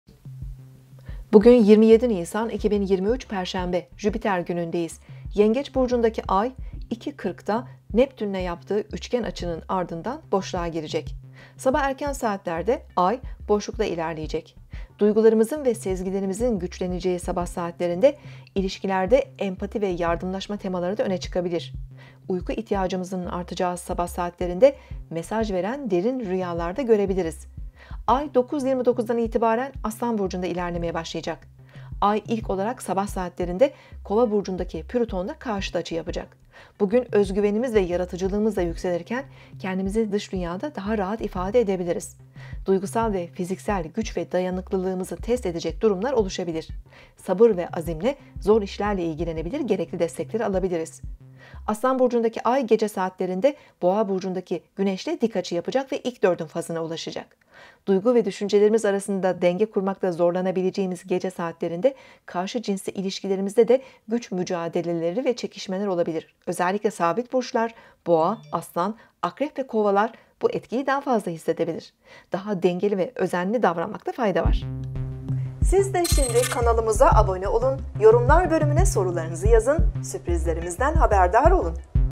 RÜYALARA DİKKAT EDELİM. 27 Nisan 2023, perşembe, günlük burç yorumu Demet Baltacı, astroloji haber
bugün 27 Nisan 2023 Perşembe Jüpiter günündeyiz yengeç burcundaki ay 2.40'ta neptünle yaptığı Üçgen açının ardından boşluğa girecek sabah erken saatlerde ay boşlukta ilerleyecek duygularımızın ve sezgilerimizin güçleneceği Sabah saatlerinde ilişkilerde empati ve Yardımlaşma temaları da öne çıkabilir uyku ihtiyacımızın artacağı Sabah saatlerinde mesaj veren derin rüyalarda görebiliriz ay 929'dan itibaren Aslan burcunda ilerlemeye başlayacak ay ilk olarak Sabah saatlerinde kova burcunla karşıda acı yapacak Bugün özgüvenimiz ve yaratıcılığımızla yükselirken kendimizi Dış dünyada daha rahat ifade edebiliriz duygusal ve fiziksel güç ve dayanıklılığımızı test edecek durumlar oluşabilir sabır ve azimle zor işlerle İlgilenebilir gerekli destekleri alabiliriz Aslan